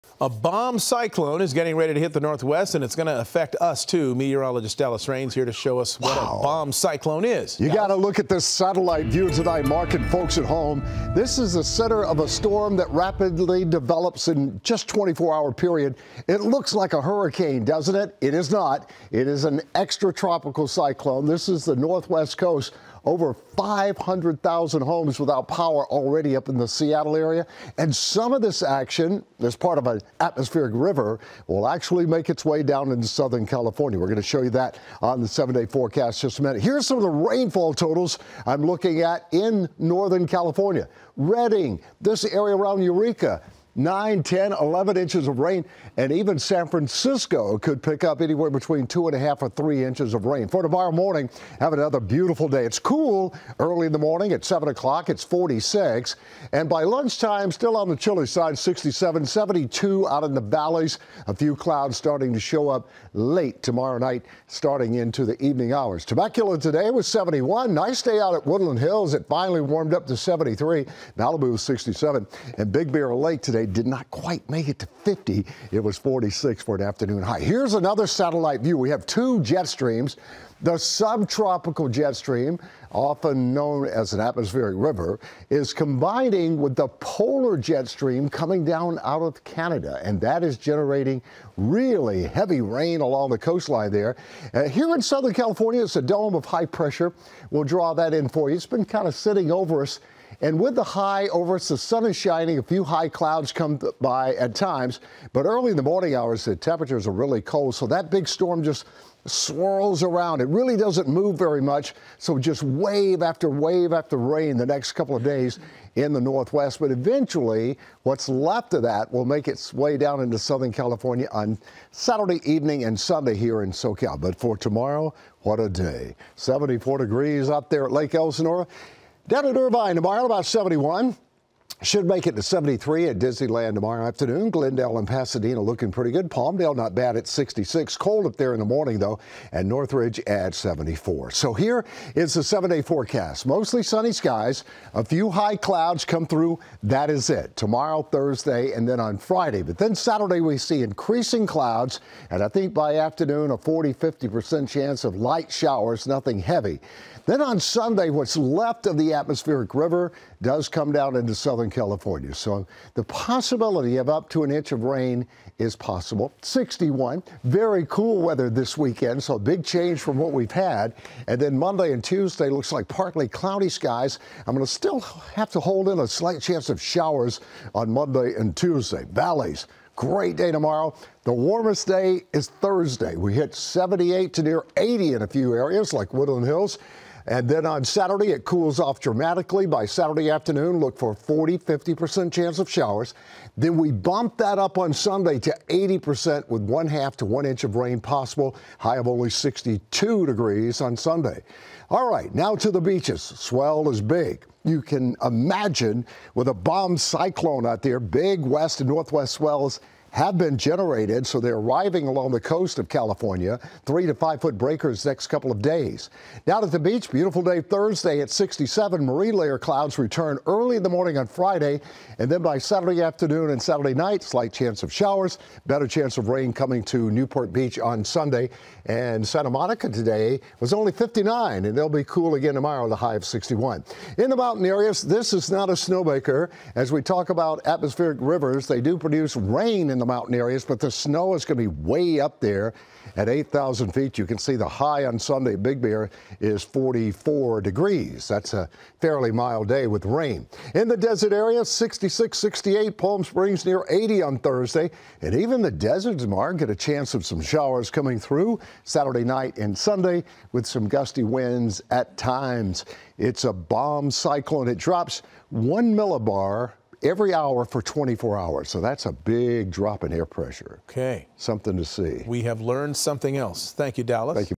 Bomb cyclone: How it will impact Southern California?
Northern California is getting deluged by an atmospheric river system. SoCal will also feel some impacts this weekend, as Dallas Raines explains.